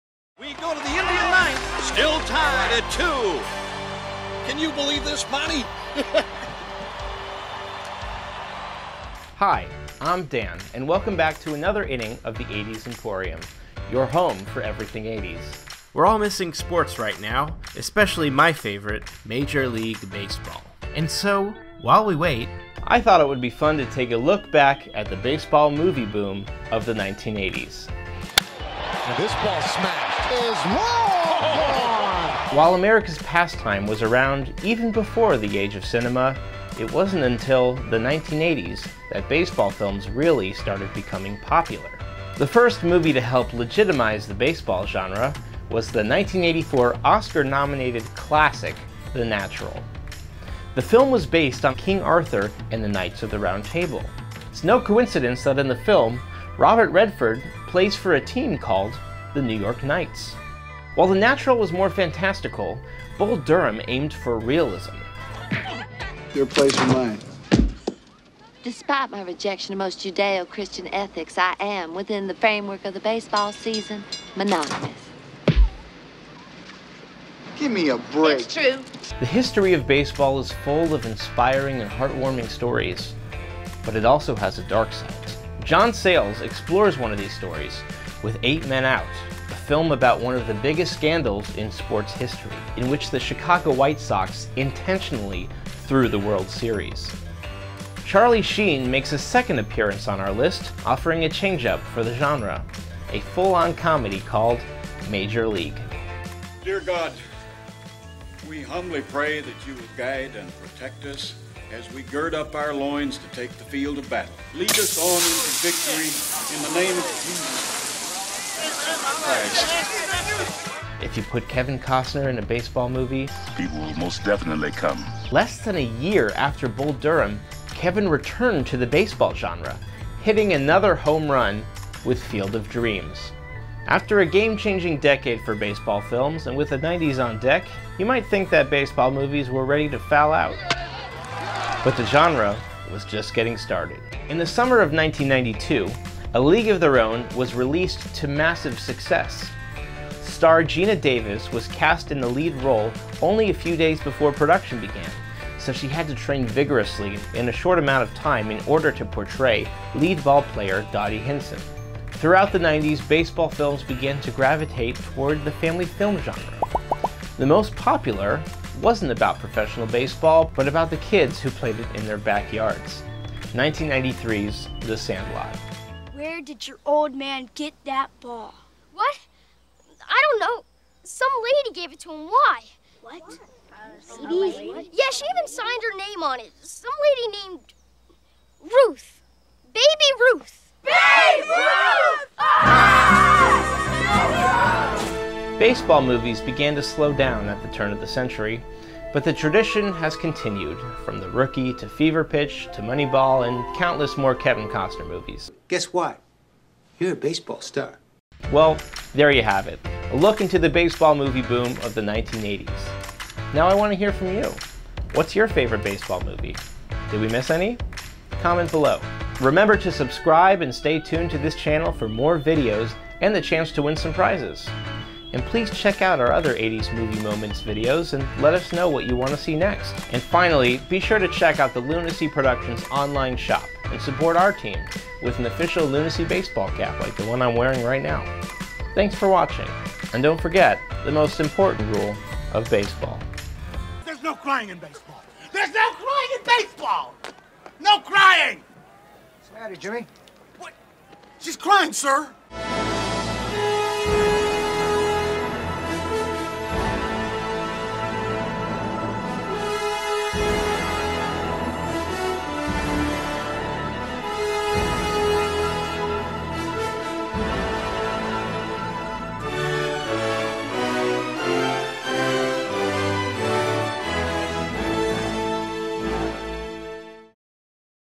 Baseball in 80's Movies (80's EMPORIUM)
In honor of the start of another season of Major League Baseball, Dan takes a trip down memory lane to the baseball movie boom of the 1980's.